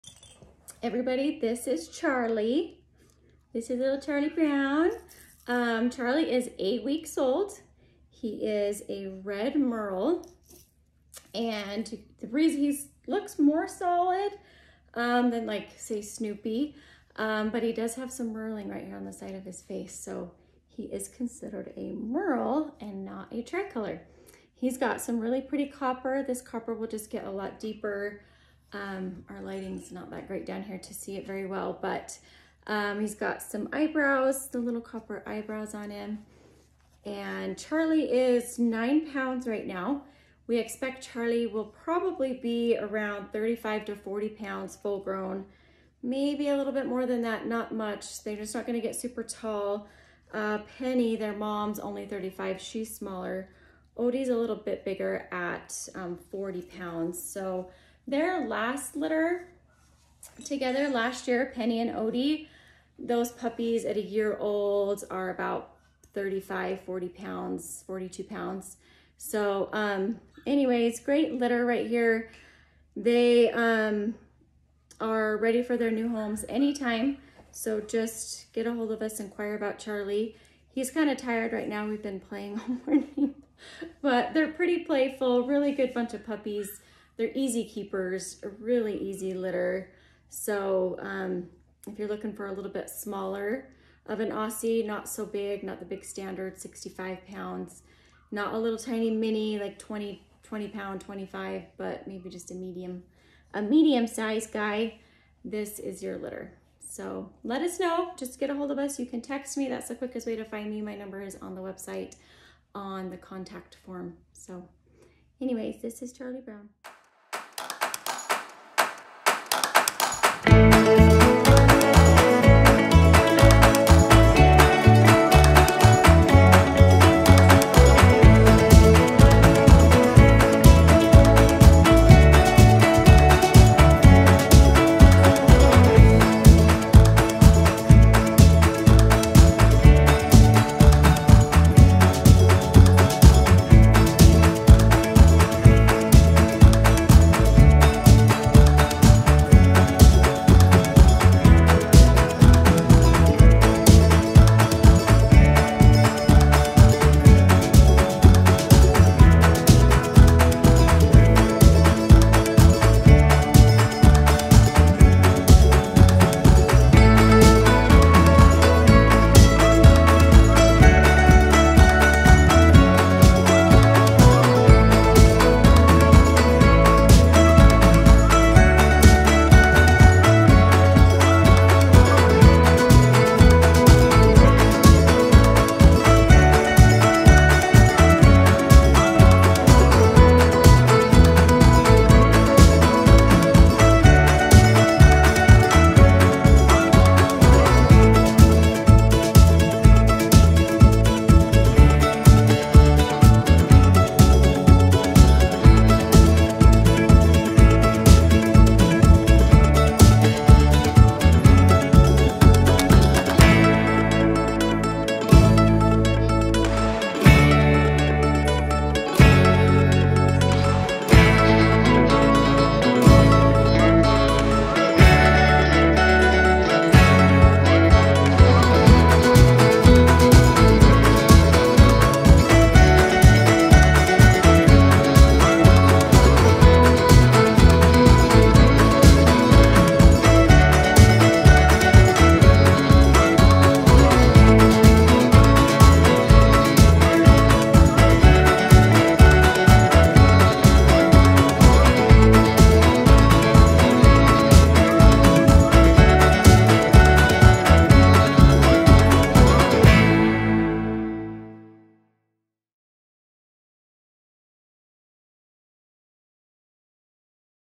Mini Australian Shepherd Puppies For Sale
Meet our beautiful mini Australian shepherd puppy for sale, Charlie Brown. He is a red merle mini Australian shepherd.  Charlie  is eight weeks old.  He has two green eyes and weighs 9 pounds.
He is beautiful red merle boy. Very Unique markings.  He has great copper points.  Charlie is very good puppy.  He is brave and so playful.  Easy going puppy! Charlie loves all the toys and loves to play on our little tykes slide.